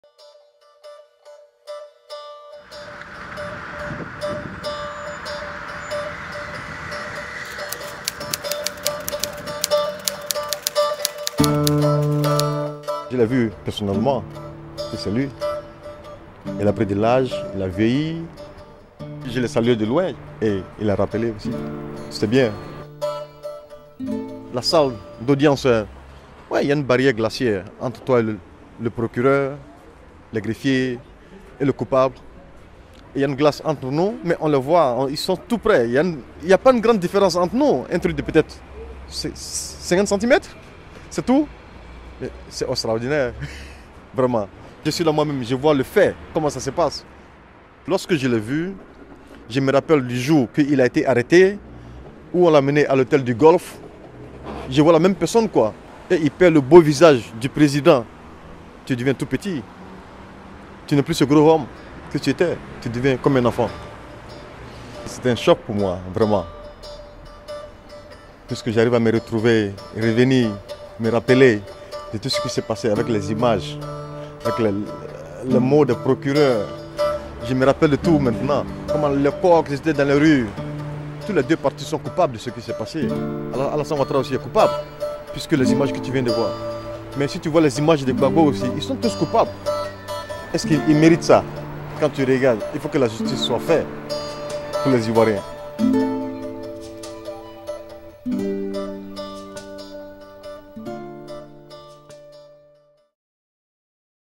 See Gbagbo at the ICC
Watch what it's like for Moussa Sylla, an Ivorian mechanic living in the Netherlands, to see his former president, Laurent Gbagbo, at the International Criminal Court (ICC). Gbagbo is waiting to hear whether he will have to stand trial for crimes against humanity during the post-election violence that swept Ivory Coast in 2010.

RNW's Africa Desk is following Moussa, once a Gbagbo supporter, as he follows the international justice case of his ex-president. This is the second episode in our video series.